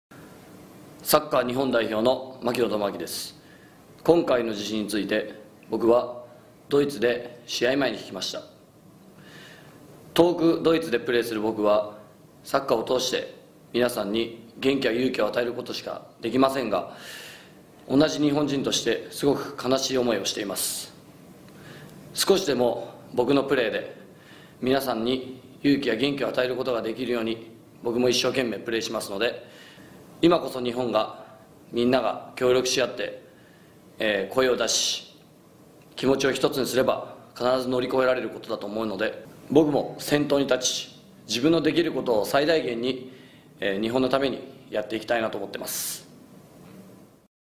ALLFOR2010! We are all together Tomoaki Makino
Video comercial da adidas sobre o terremoto do japão, estrelando tomoaki makino